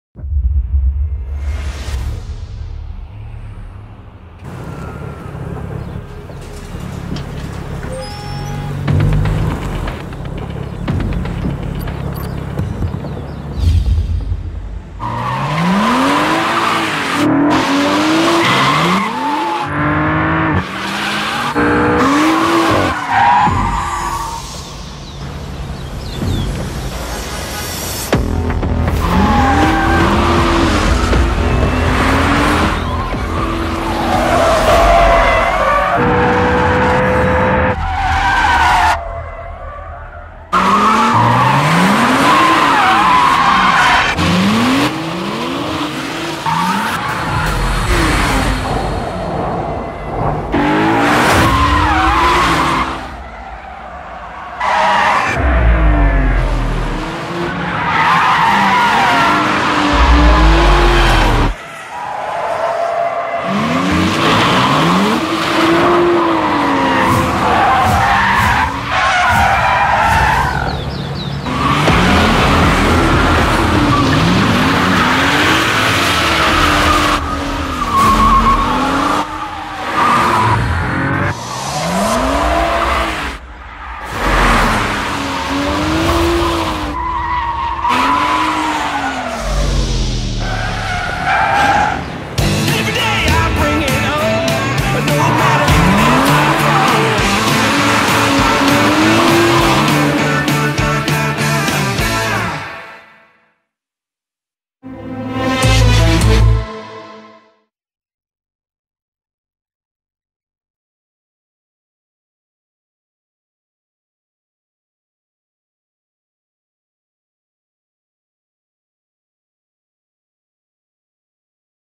BMW - The Epic Driftmob feat BMW M235i - CHECK OUT THIS CRAZY VIDEO
Is it possible to make a drift-choreography in a rotary traffic? We took some hollywood stunt driver and added the same amount of BMW M235i cars. The result: About 90 seconds of driving pleasure and smoking tyres. Please note that all these driving-scenes are real. To avoid accidents, we had to block the road.